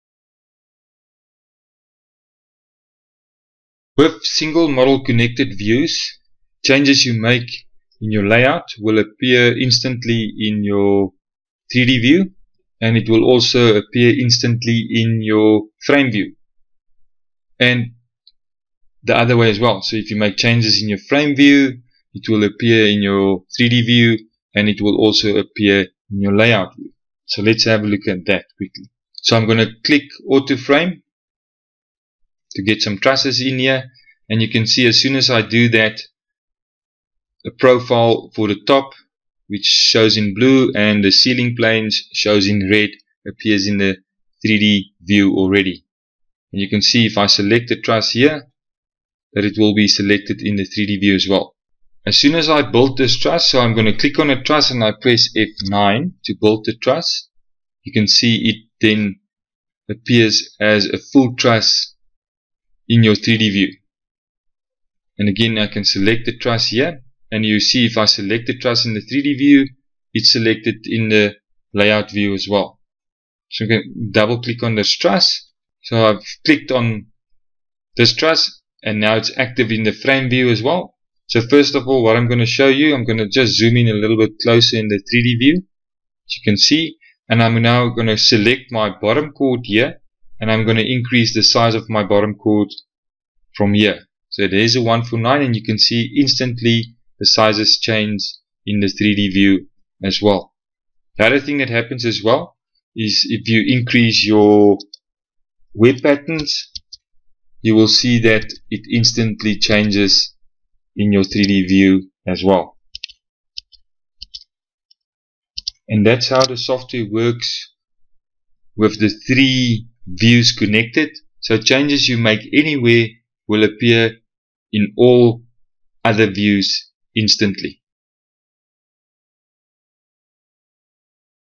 MiTek PAMIR SA - Connected Views
ROOF TRUSS DESIGN SOFTWARE

MiTek PAMIR combines roof layout, truss engineering, high-quality CAD output, and 3D tools into a single fully integrated application. PAMIR enables quicker estimating capabilities, faster roof-editing tools, and a more user-friendly software environment, specifically designed for modern truss manufacturers.

PAMIR is the next generation of prefabrication software. It is more dynamic, more powerful and more versatile than any other software package available.

MiTek PAMIR - THE FUTURE OF FRAMING